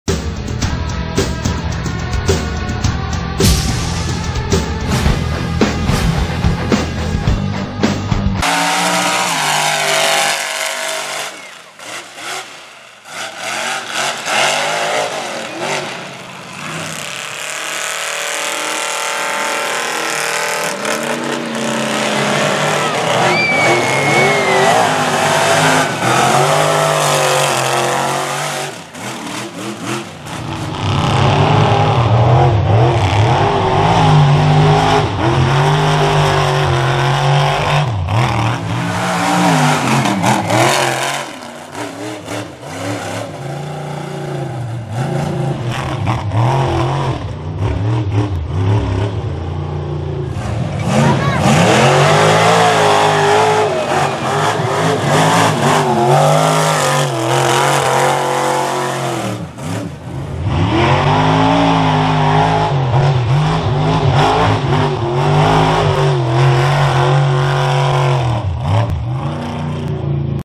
Perkins Having Fun At Bentley Lake Road Mud Bog
Mudding action from Bentley Lake Road Mud Bog Dec 2015